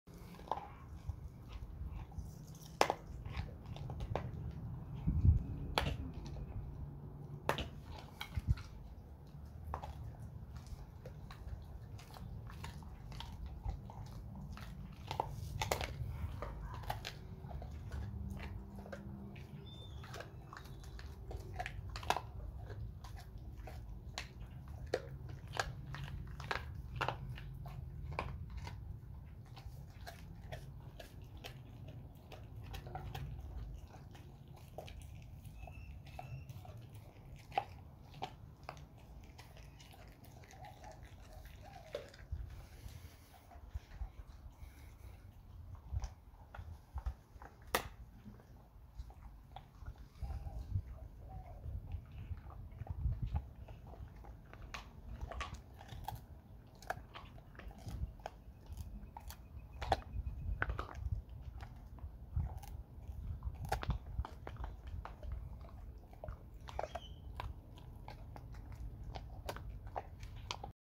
Chomp, chomp, chomp...mmmm good! 🐶🥩
The sounds of my girls enjoying beef bones from Charley's steakhouse! charlotte star rescuedog rescue sharpei pitbull steak bones dog 🥩🐶🐶🥩